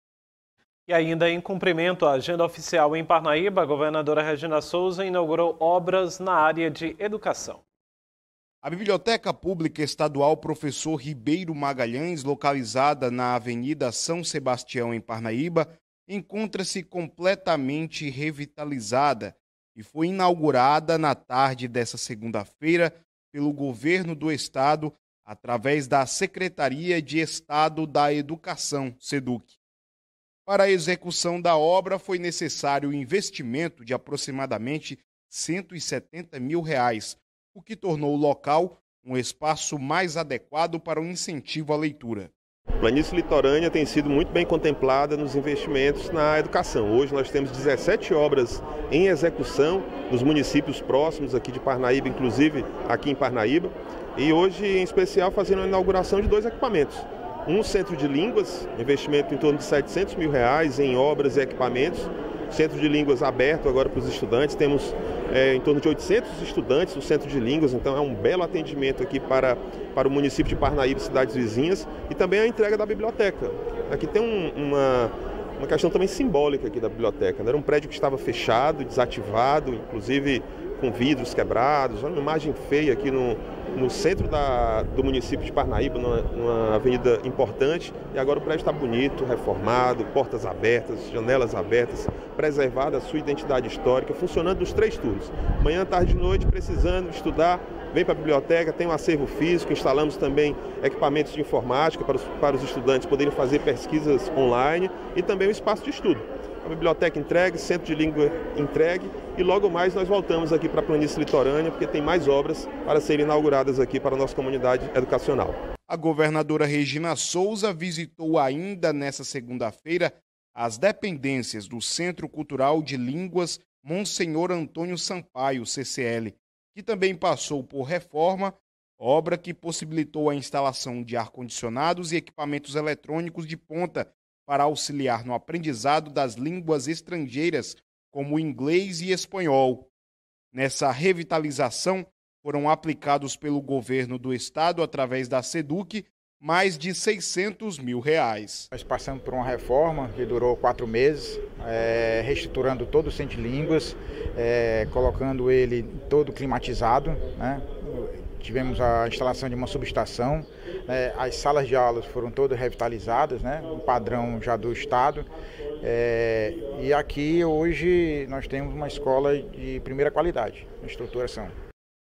Governo do Piauí conclui revitalização de Centro de Línguas e Biblioteca
A Biblioteca Pública Estadual Professor Ribeiro Magalhães, localizada na Avenida São Sebastião em Parnaíba, encontra-se completamente revitalizada, e também foi inaugurada na tarde dessa segunda-feira pelo Governo do Estado, através da Secretaria de Estado da Educação (SEDUC).
Regina Sousa ainda visitou as dependências do Centro Cultural de Línguas Monsenhor Antônio Sampaio (CCL), que passou por reforma – obra que possibilitou a instalação de ar condicionados e equipamentos eletrônicos de ponta para auxiliar no aprendizado das línguas estrangeiras, como o inglês e espanhol.